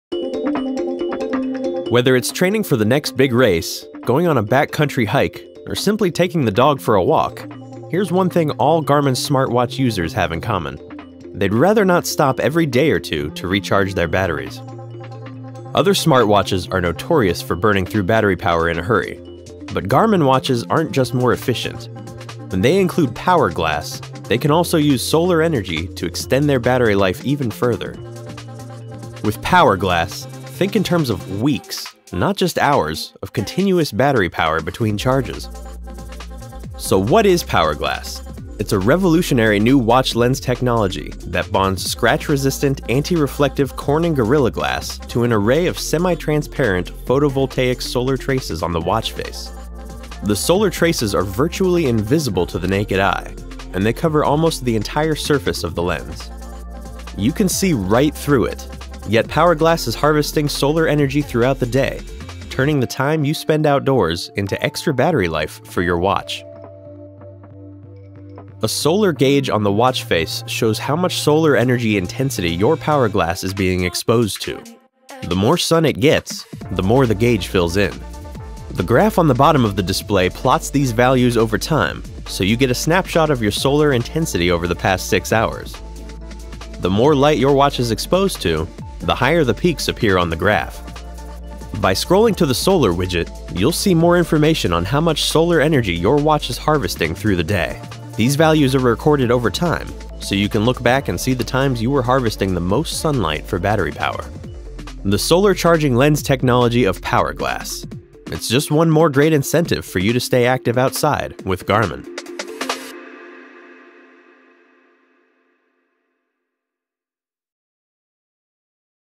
Garmin: The long-running advantages of Power Glass solar charging
See how harnessing the sun’s energy with Power Glass™ solar charging lenses works to greatly extend battery life and on-wrist usability for select Garmin sportwatches and other wearable devices.